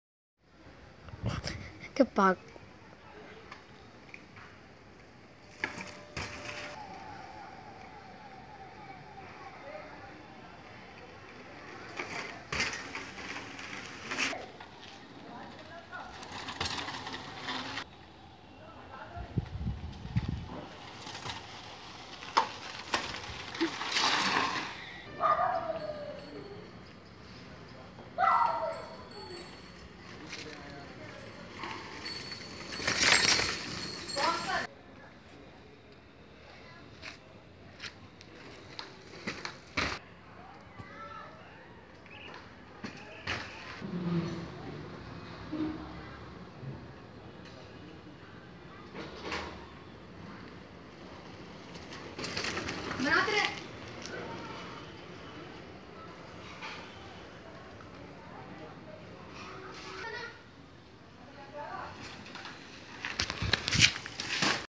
Simple cycle stunts
A boy doing a good cycle stunts in home